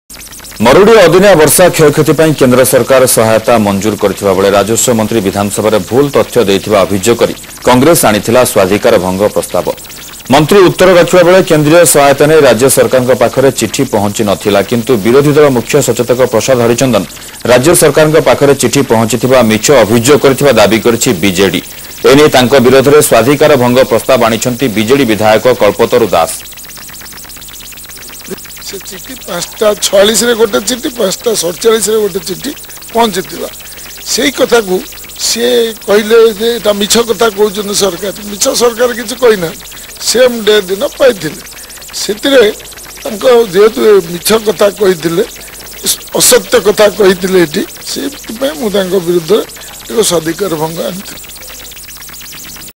Kanak TV Video: War of words between BJD-Congress MLA in Odisha Assembly
There is a war of words between Biju Janata Dal MLA Kalpataru Das and Congress MLA Prasad Harichandan in Odisha Assembly on Central funding to the state.